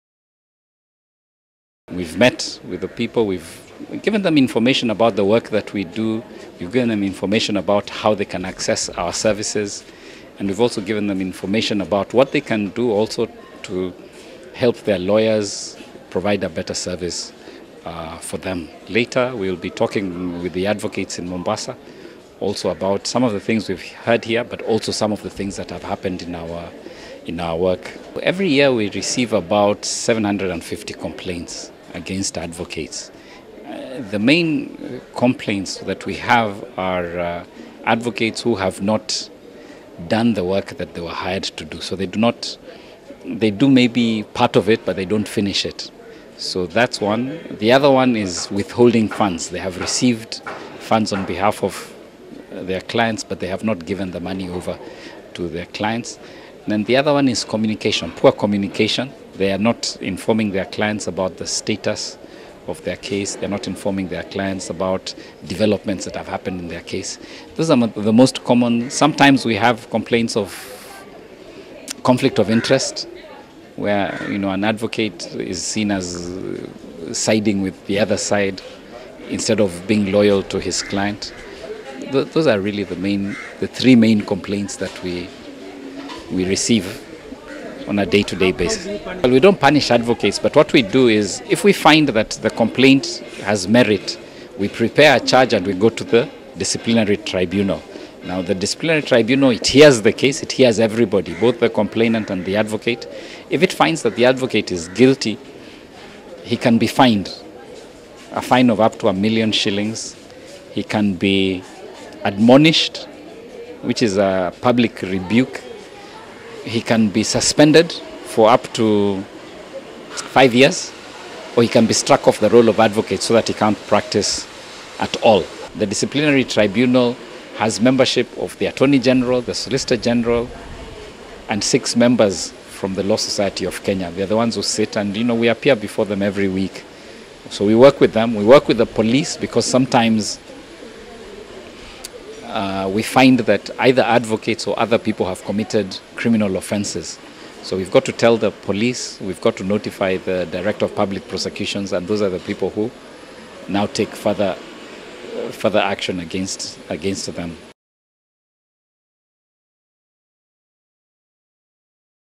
Alarm as number of rogue lawyers goes up
Complaints against rogue lawyers are on the rise, the Advocates Complaints Commission (ACC) has said. Every year, ACC receives about 750 complaints against lawyers. Speaking in Mombasa, ACC chairperson Beauttah Siganga said the complaints were mainly about lawyers’ failure to fulfil obligations, holding clients’ compensation and poor communication.
Speaking in Mombasa during a legal aid sensitisation meeting at the Kenya School of Government, Mr Siganga urged advocates to handle their work with integrity and uphold professionalism and honesty.
He warned them against swindling the public of their hard-earned money.